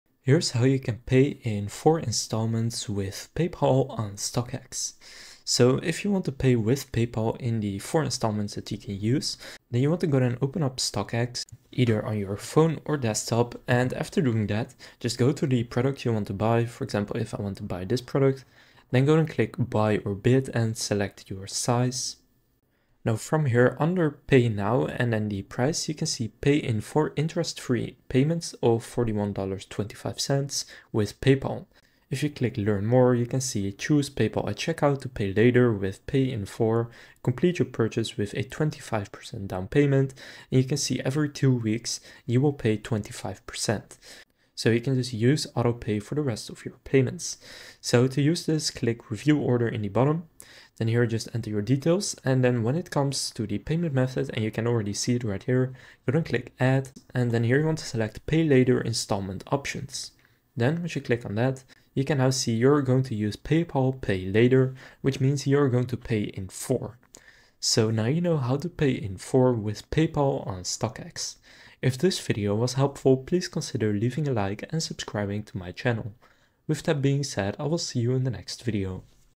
How to Pay in 4 With PayPal on StockX - Step by Step
In this video I'll show you how to Pay in 4 With PayPal on StockX.